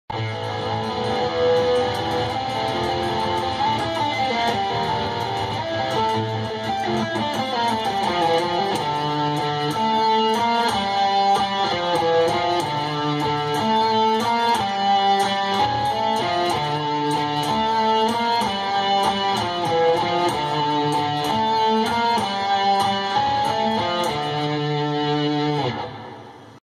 ÆRA - Riffs #1
Brief playing of a couple of riffs recorded for the upcoming album.